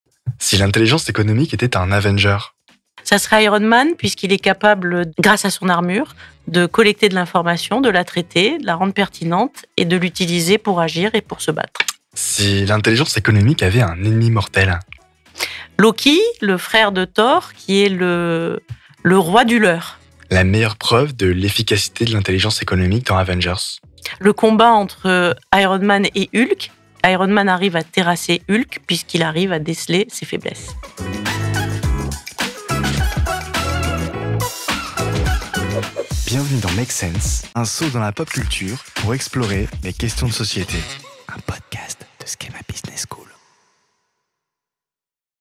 Teaser Épisode 15: Iron Man est-il le plus puissant des Avengers parce qu’il est le mieux informé ?
Tony Stark, alias Iron Man, n’est qu’un homme. Pourtant, il arrive à dompter Hulk, Captain America et se permet d’être le boss des Avengers, sa compagnie de super-héros. Son pouvoir ? Il est parfaitement renseigné, il connaît TOUT de ses ennemis. Dans le monde réel, on appelle ça l’intelligence économique (IE).

Comment acquérir ce pouvoir ? Dans quels buts, plus ou moins obscurs, l’utilisent les Etats, les grandes entreprises, les collectivités ? Comment identifier la bonne information dans un monde surchargé de messages ?

Pour se bâtir une armure digne de celle d’Iron Man, Kevin Erkeletyan a fait appel à l’Iron Woman de SKEMA Business School : sa directrice générale Alice Guilhon. Fan de l’univers de Marvel et auteur d’un livre de référence sur l’Intelligence économique, elle nous donne toutes les clés pour livrer bataille dans ce monde incertain.

Tony Stark, aka Iron Man, is just a man. Yet he manages to tame Hulk and Captain America and even becomes the leader of the Avengers, his team of superheroes. His power? He is perfectly informed; he knows EVERYTHING about his enemies. This is known as economic intelligence (EI) in the real world.

How can one acquire this power? For what purposes—some clear, others perhaps more obscure—do states, major corporations and communities use it? How do you identify the right information in a world overloaded with messages?

Kevin Erkeletyan turned to the Iron Woman of SKEMA Business School, its Dean, Alice Guilhon, to build armour worthy of Iron Man. A Marvel fan and author of a renowned book on economic intelligence, she shares all the keys to fighting in this uncertain world.